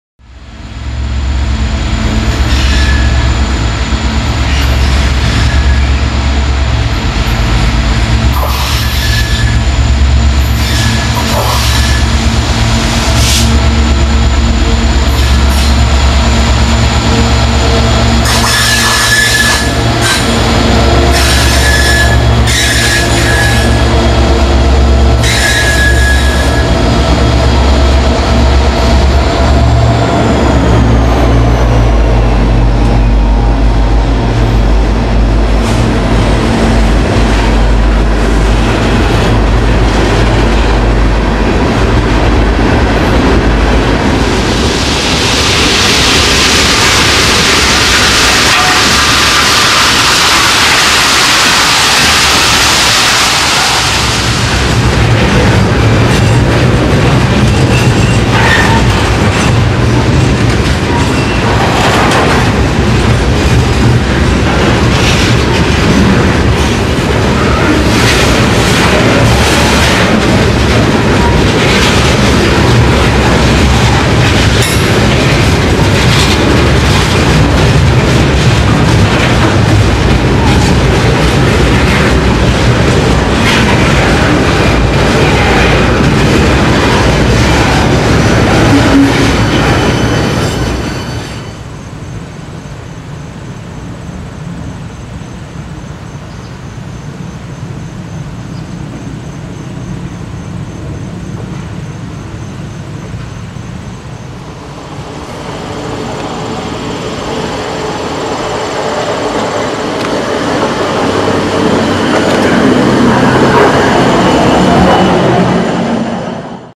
NS 921/Herzog Ballest Train Northampton, PA
During the middle of August 2011, Norfolk Southern symbol 921, a Herzog Ballest train made its way north along the Upper Lehigh Line as part of NS's Summer MOW work. Here we'll see the 921 at MP 94 in Northampton, PA behind a trio of SD40E's.